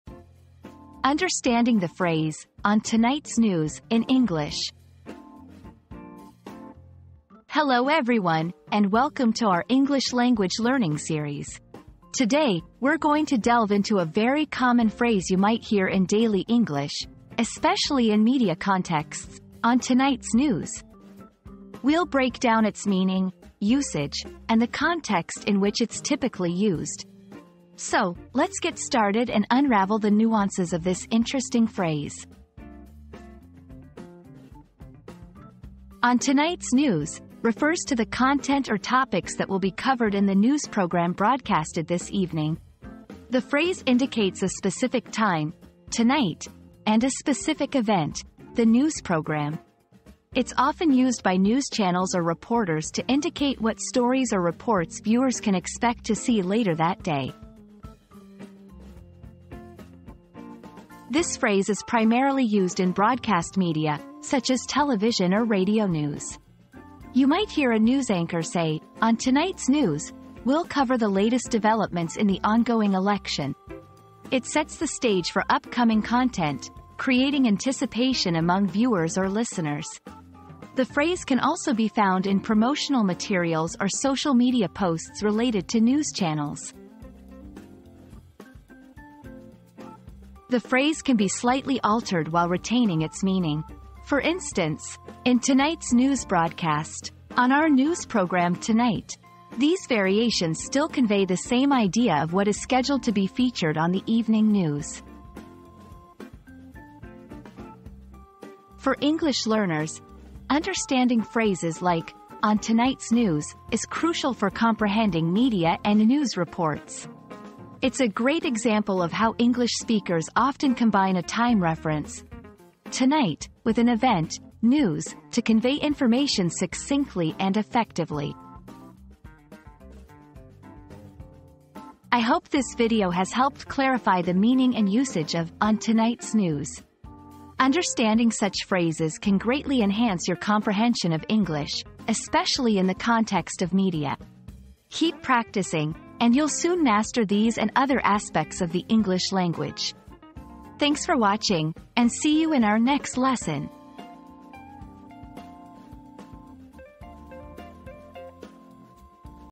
Understanding the Phrase "On Tonight's News" in English
Unraveling the Mystery: On Tonight's News Explained • Discover the meaning and usage of the commonly heard phrase 'On Tonight's News' in English. Dive into the fascinating world of news broadcasting and unravel the secrets behind this intriguing phrase that keeps us informed and engaged.

00:00 • Introduction - Understanding the Phrase "On Tonight's News" in English
00:35 • The Basic Meaning
01:00 • Contextual Usage
01:29 • Variations of the Phrase
01:48 • Importance in Learning English